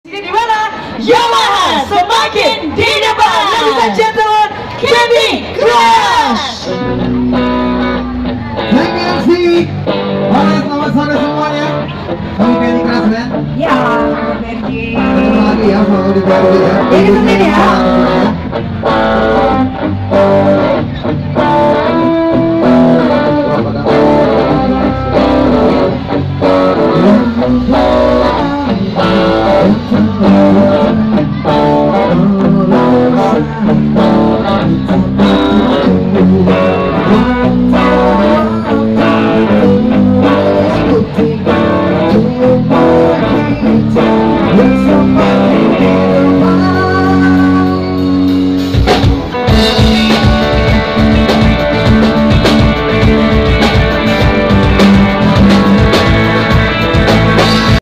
Ovpro, candy crush band, with mc yesica & agung
At pekan raya jakarta ji-expo 2019
Uploaded by: ovpro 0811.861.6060 (call/wa)
Stand booth yamaha
Band top 40 allround & hits
Available all event